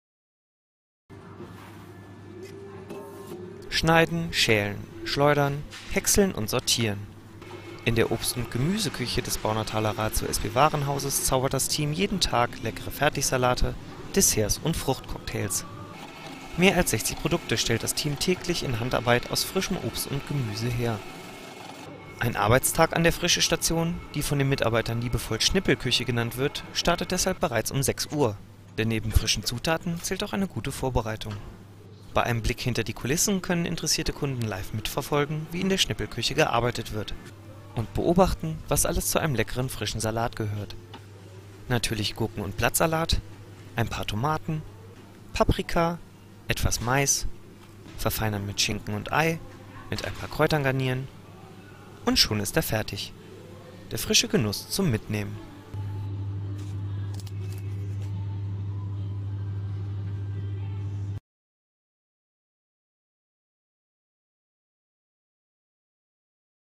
Zu Gast in der RATIO-Schnippelküche
Mehr als 60 Salate, Desserts und Fruchtcocktails zaubert das RATIO-Team täglich aus frischem Obst und Gemüse.